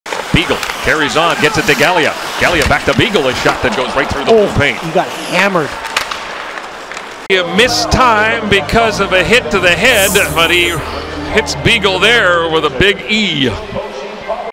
Gotta See It: Korpikoski not penalized for elbow to Beagle's head
Watch as Lauri Korpikoski catches Jay Beagle with an elbow to the head in the first period.